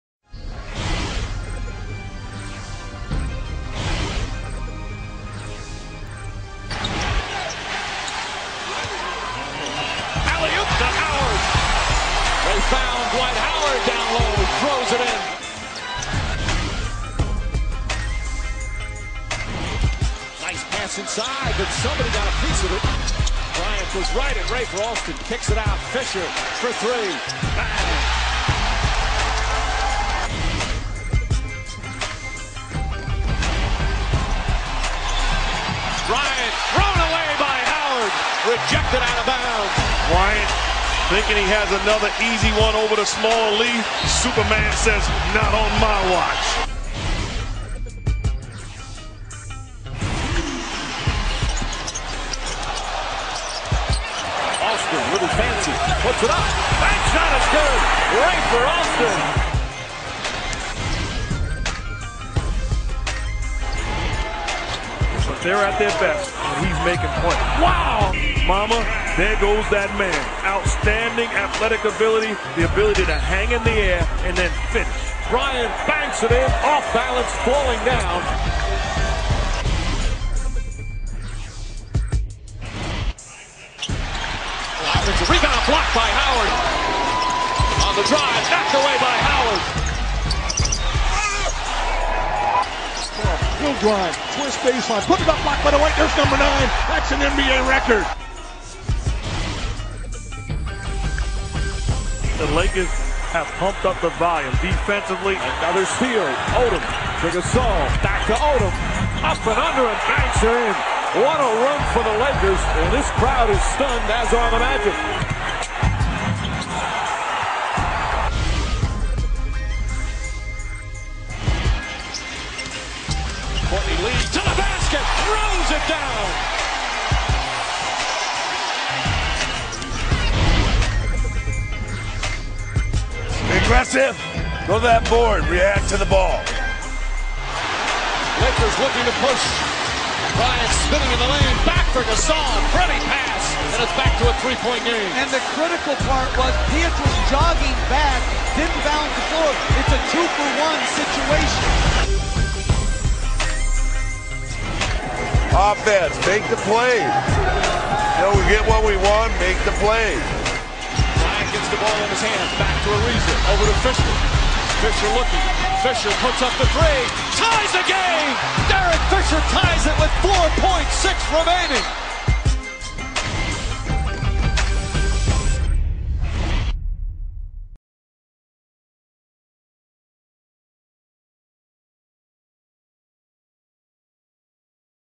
Top 10: 2009 NBA Finals
for more highlights.  The Los Angeles Lakers have won the 2009 NBA Finals, check out the Top 10 Plays from the 2009 Finals. Top 10: 2009 NBA Finals